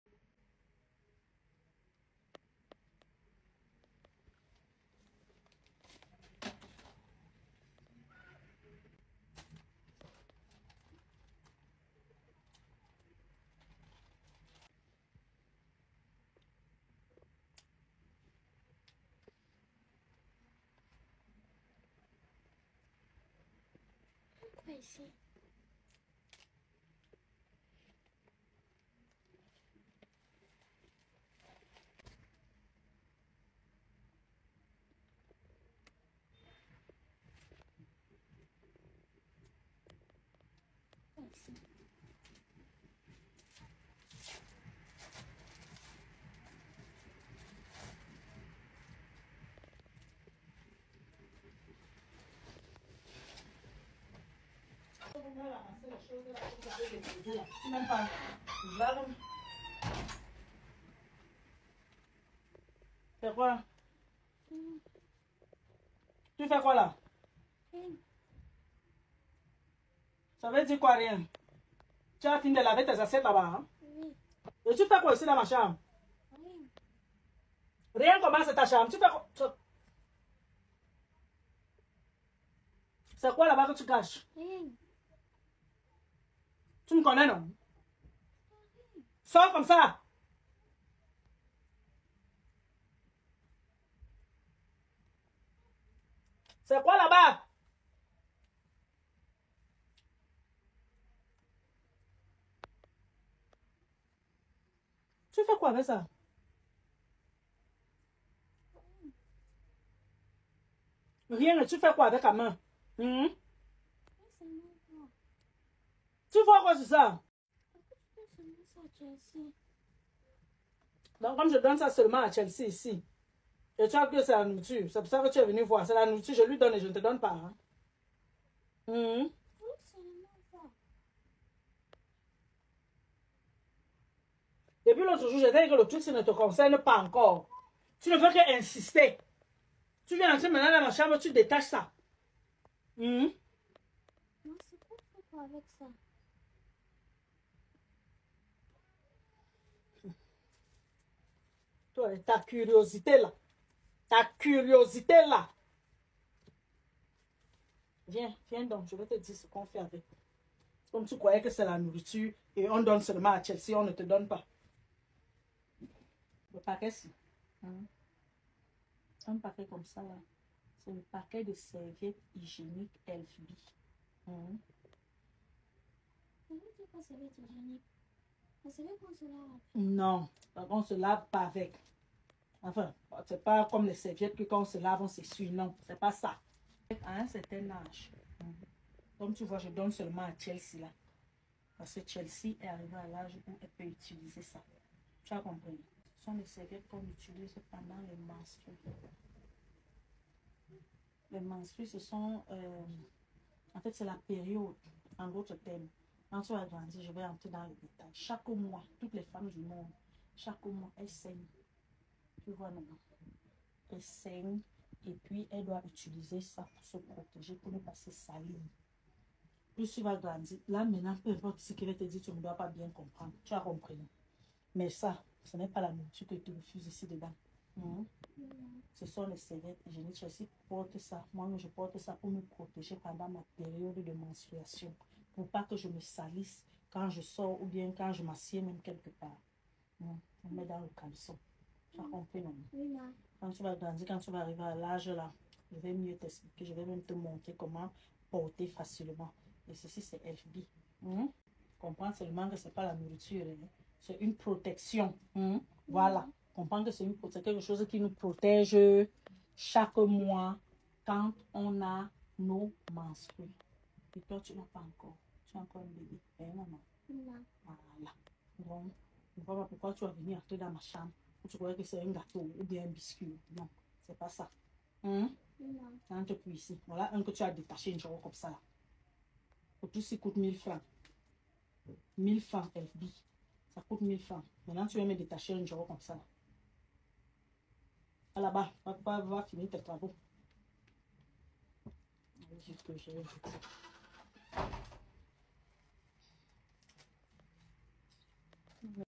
Divine et les serviettes hygiéniques
Divine 😂😂😂 tu es pressée pour rien mon bébé , monte descend le moment viendra où ça sera ton tour d’utiliser les serviettes hygiéniques HELFBI.cm pour le moment ça ne te concerne pas encore wokoooo